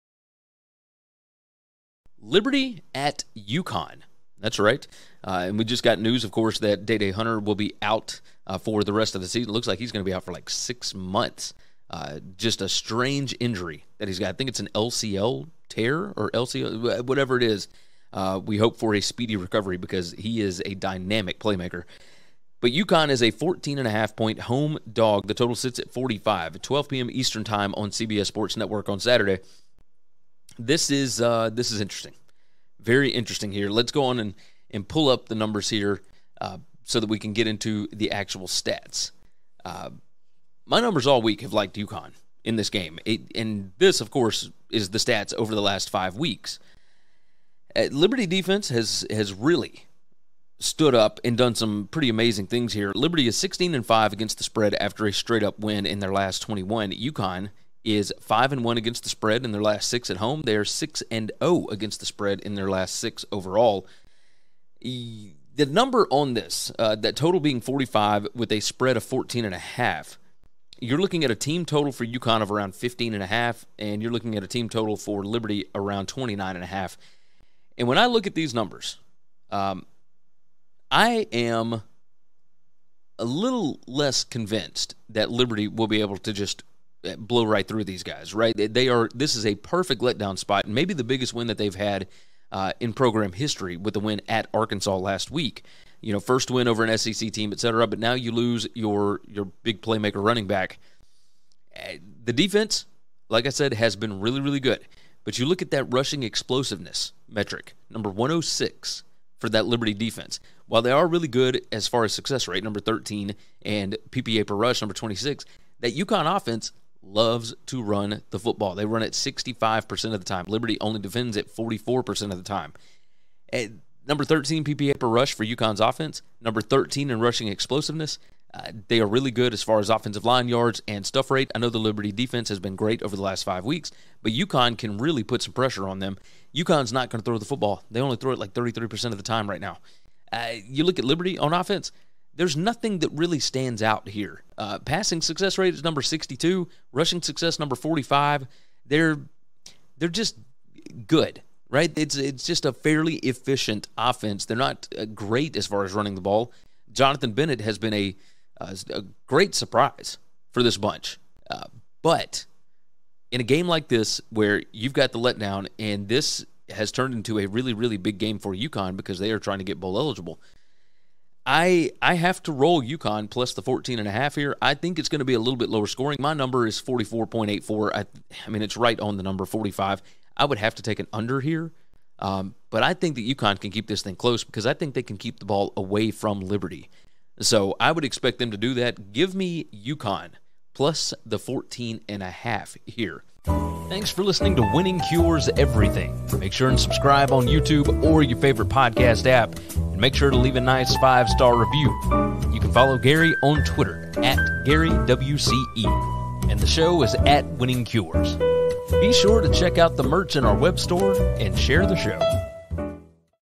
Liberty vs UConn Picks & Prediction Against the Spread 2022 College Football Analysis
Liberty vs UConn Picks Against the Spread 2022 Predictions

Gary gives a quick preview, analysis, prediction & his pick (lean) against the spread (ATS) for the 2022 College Football Week 10 matchup featuring UConn vs Liberty!